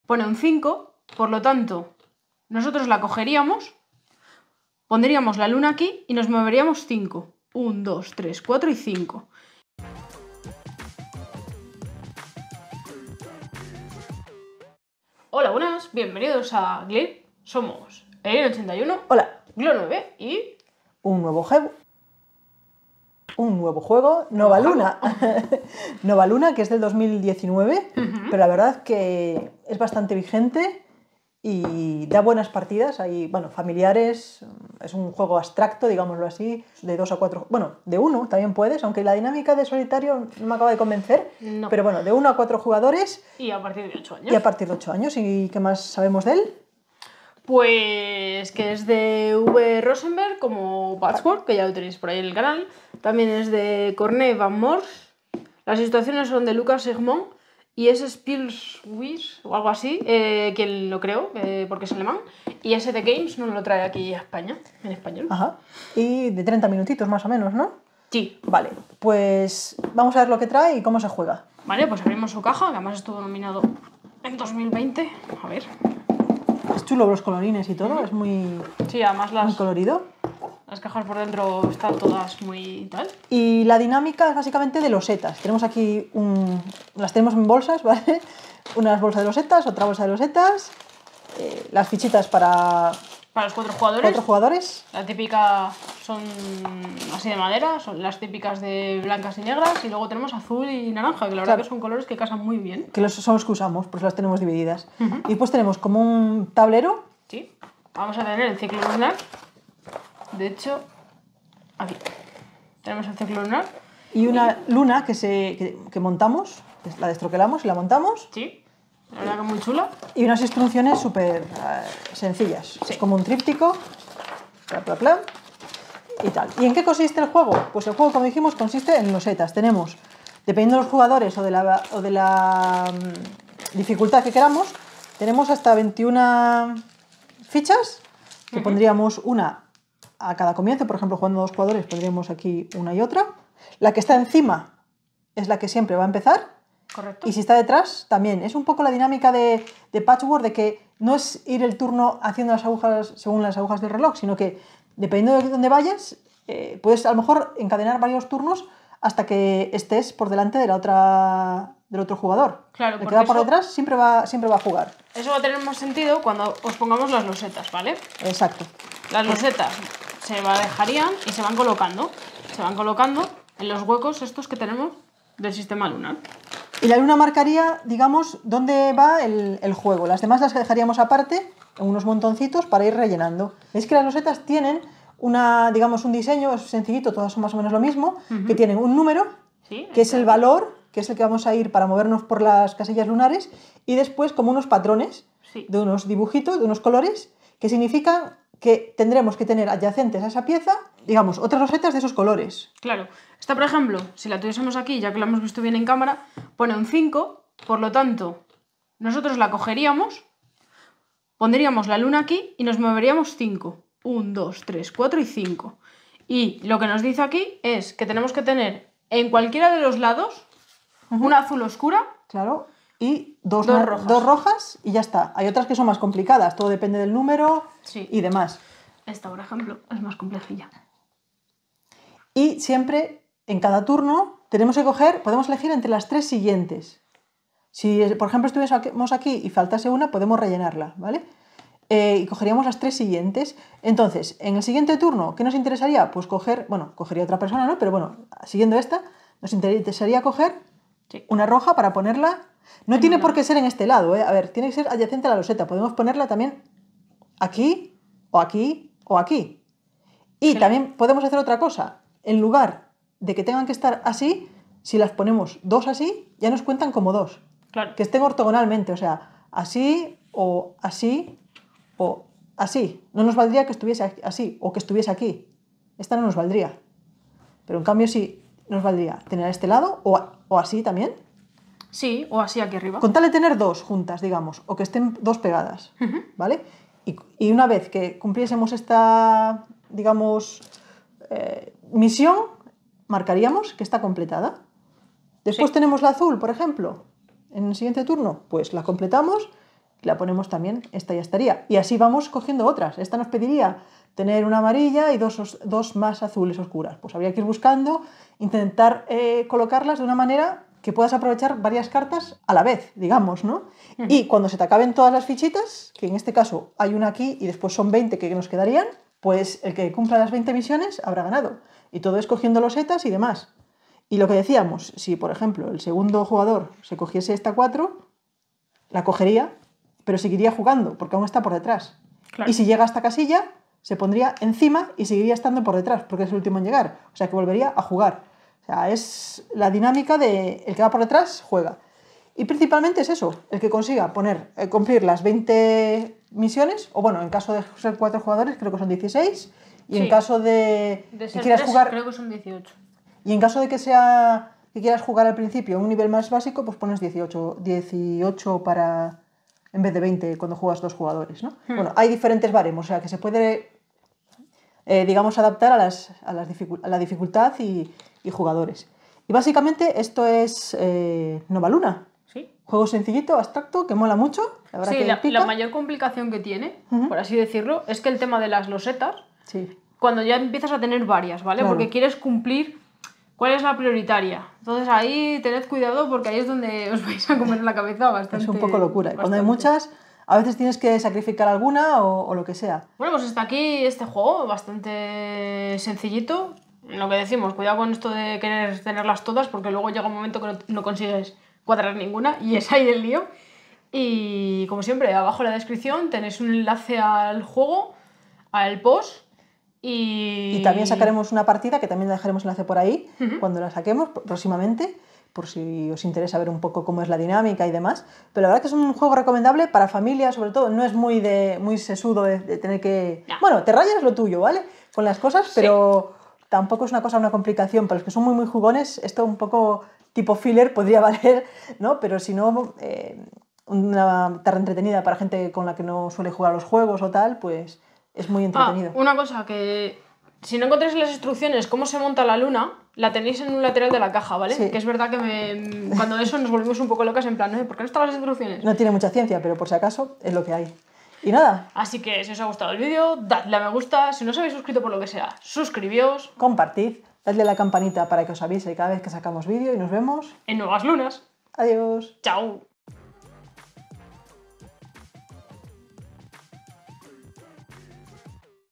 Nova Luna, todo lo que necesitas saber de este juego de mesa 👥
Nova Luna es un juego de mesa abstracto basado en el ciclo lunar. Prepárate para cumplir con todas las tareas de las losetas para ganar la partida.

Cómo conseguir el juego Nova Luna:
- Nova Luna en Amazon

👥 1-4 jugadores
⌛ 30 minutos
🧸 + 8 años
⚙ Edition Spielwiese / SD Games

0:00 Introducción
0:15 Presentación Nova Luna
1:12 Unbox del juego Nova Luna
2:20 Cómo se juega a Nova Luna
6:33 Opinión Nova Luna

PRODUCCIONES GLIRP.